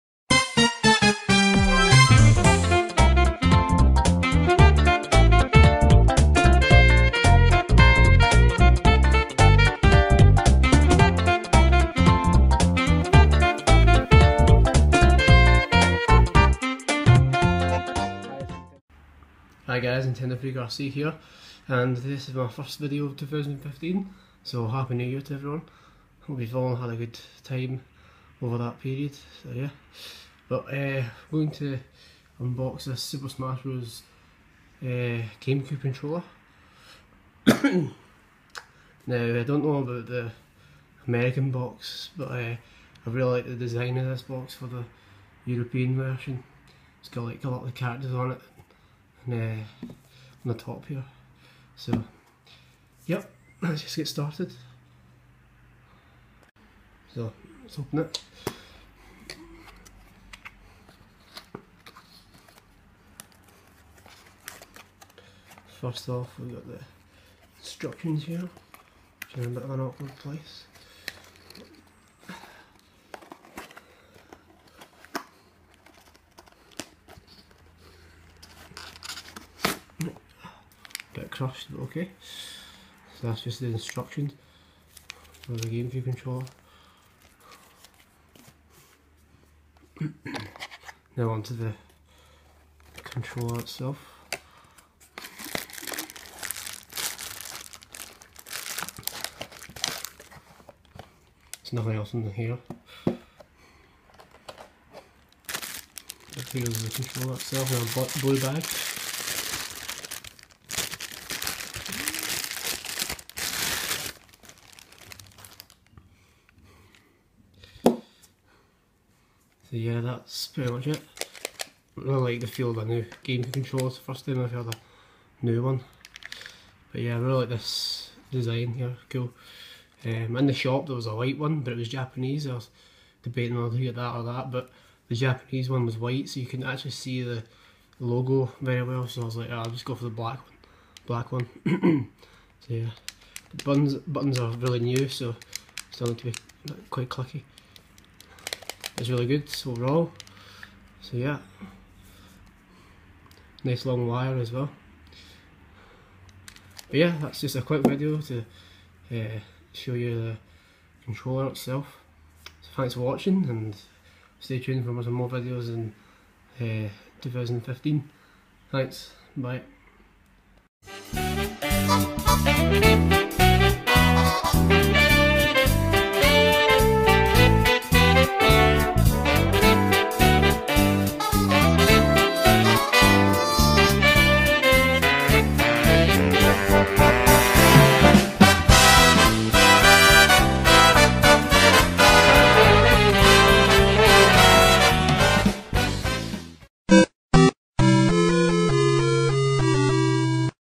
Super Smash Bros. GameCube Controller Unboxing
Here's my unboxing of the Super Smash Bros. Gamecube Controller this cost me £29.99, were as in a different game shop they were wanting £39.99 for it! So I got a good deal here.